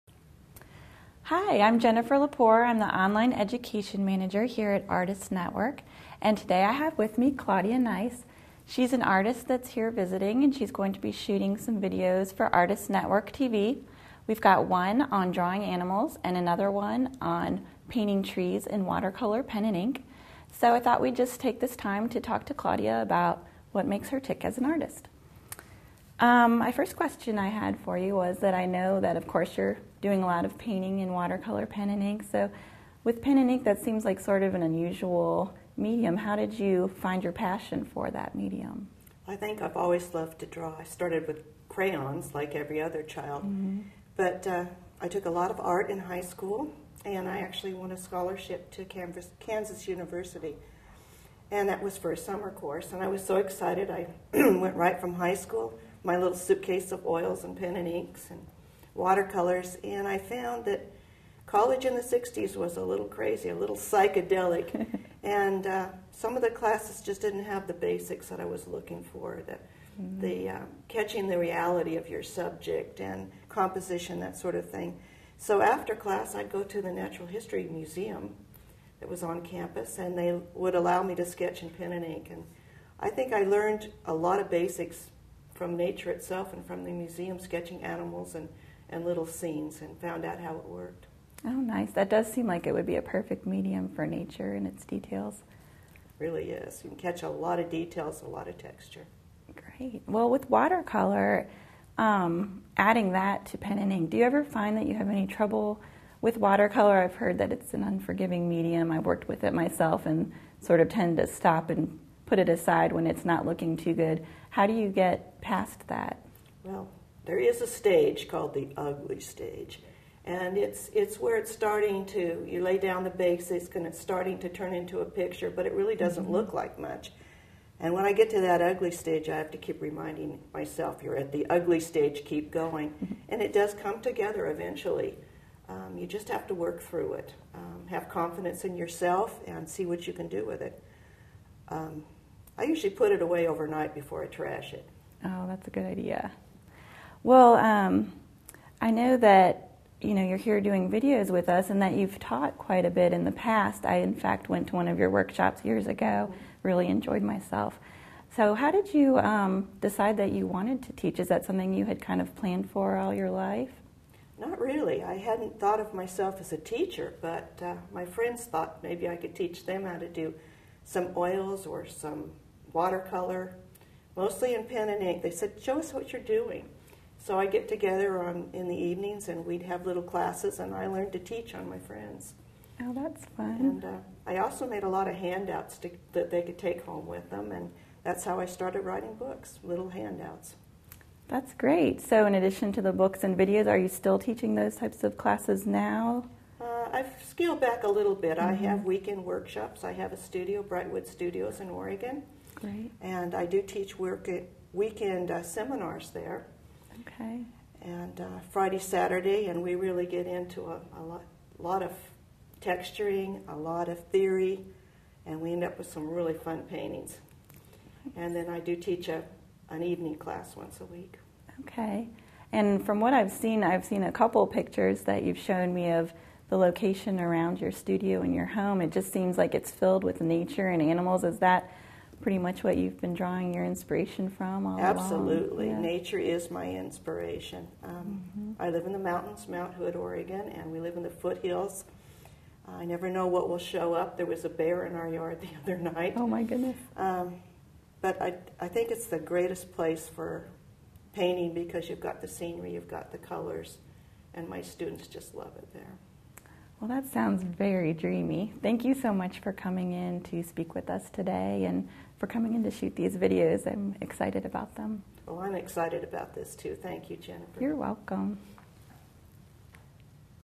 ArtistsNetwork.TV Interview with Claudia Nice
Claudia Nice is a native of the Pacific Northwest and a (mostly) self-taught artist who developed her realistic art style by sketching from nature. Her oils, acrylics, watercolors and drawings can be found in private collections nationally and internationally.

An author of more than twenty-five successful art instruction books, Claudia has traveled internationally conducting workshops, seminars and demonstrations at schools, clubs, art shops and trade shows, and has served as a judge at major art competitions across America.

Claudia operates her own teaching studio, Brightwood Studio in the beautiful Cascade Wilderness near Mt. Hood Oregon.

Now she joins ArtistsNetwork.TV for two video workshops: Painting Trees in Watercolor, Pen & Ink and Keys to Drawing Realistic Animals (available soon through

In this interview, Claudia talks with ArtistsNetwork.TV's, Jennifer Lepore, about her paintings and inspirations.